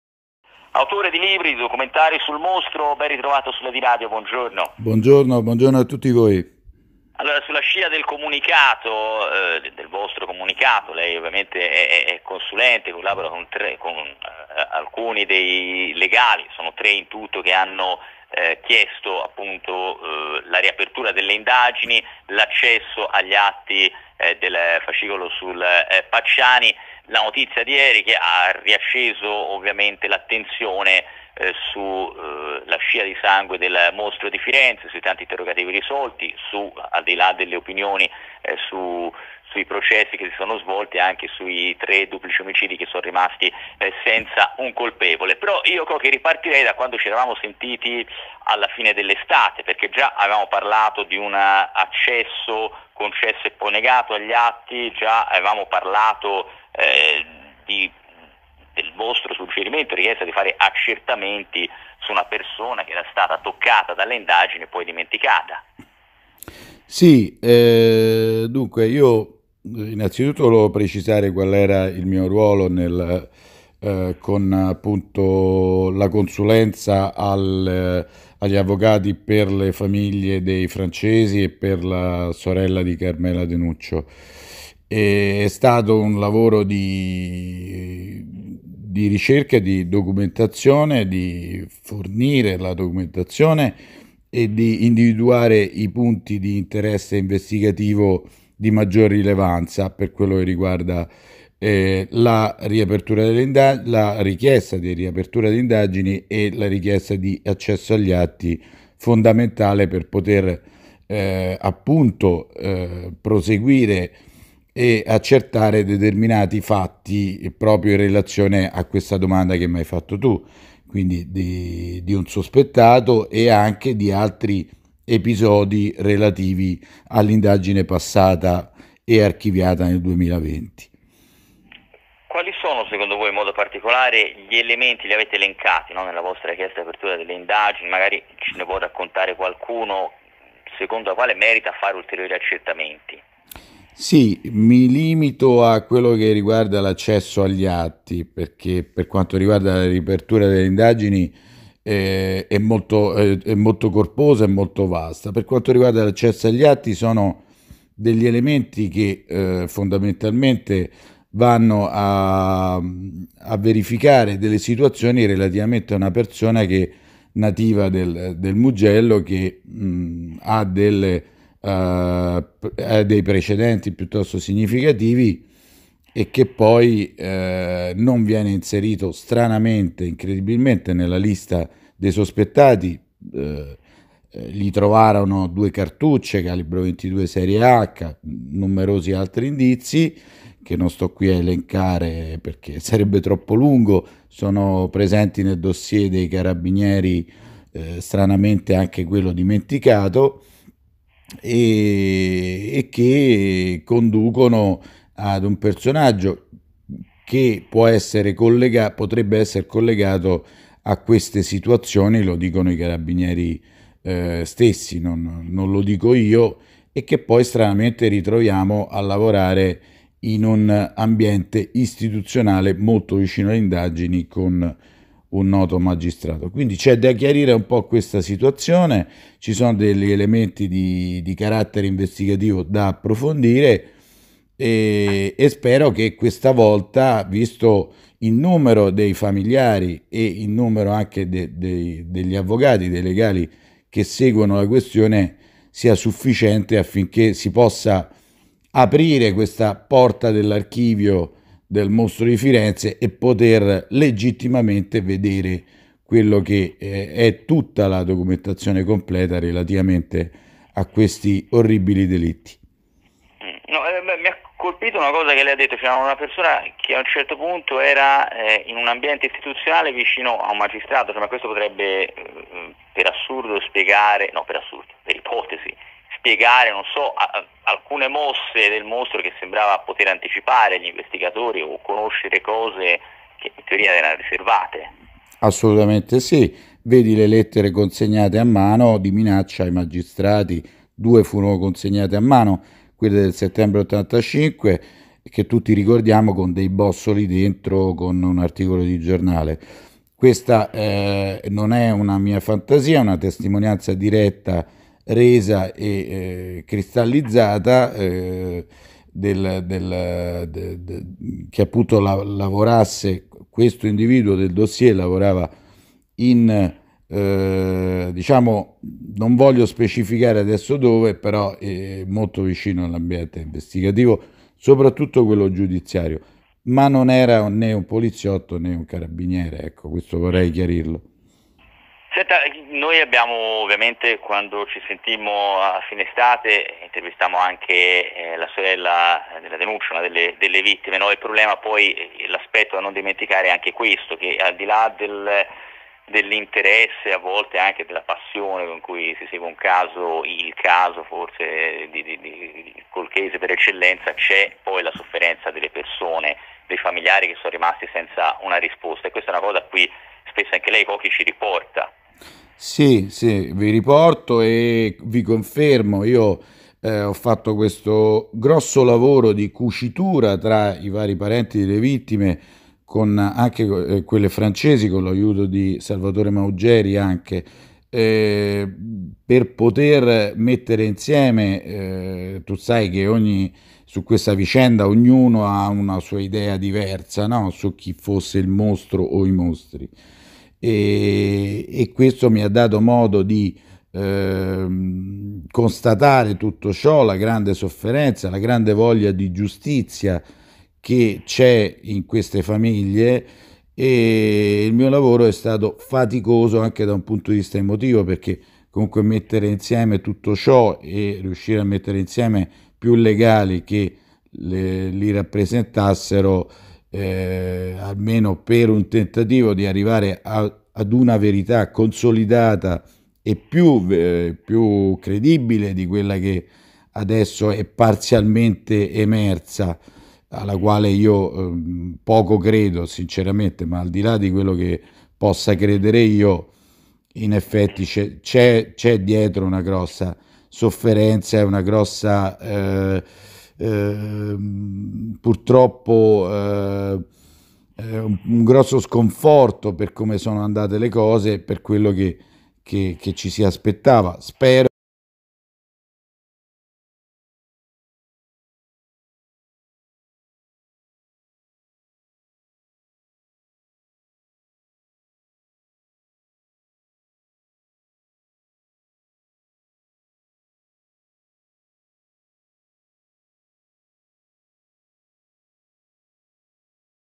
mostro di firenze - richiesta atti e riapertura indagini
ladyradio 24.3.22 di Francesco Pini
con Paolo Cochi
nuove indagini e richiesta di riapertura del caso.
i familiari delle vittime e gli avvocati di parte civile